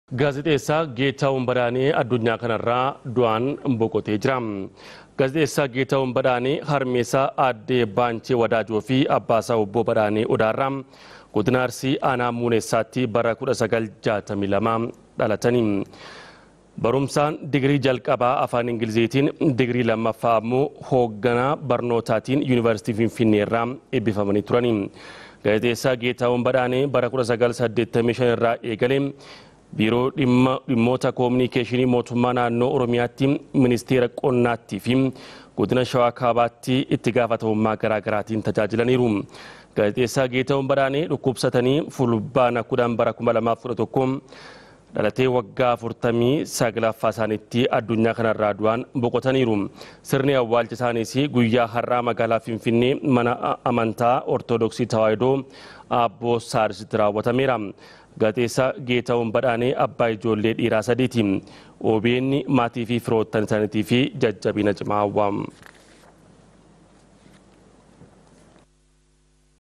Oduu Gaddaa obbo Geetaahun Badhaanee addunyaa kanarraa du'aan boqotan. 11 01 2011
Oduu Gaddaa
[OBN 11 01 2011] Biiroo Beeksisaafi quunnamtii Oromiyaatti gaazexeessaa kan turan obbo Geetaahun Badhaanee addunyaa kanarraa du'aan boqotan.
Obbo Geetaahun hanga lubbuunisaani du'aan boqotanitti Biiroo Dhimmoota Kominikeeshinii Mootummaa Naannoo Oromiyaatti Daayireektara Odiitii keessaa ta'uun tajaajilaniiru.
Sirni awwaalchisaanii guyyaa har'aa Finfinnee kutaa magaalaa Yakkaa bataskaana Abbootti bakka firoottaniifi hiriyyoonni isaanii argamanitti raawwatameera.
Obbo Geetaahun Badhaanee dhalatanii umrii waggaa 49tti addunyaa kanarraa du'aan boqotaniiru.
Obbo Getaahun Badhaanee Abbaa maatii fi Abbaa ijoollee dhiira Sadi turan.
Biiroon Dhimmoota Kominikeeshinii Mootummaa Naannoo Oromiyaa gaddi itti dhagayame guddaa ta'uu ibsaa.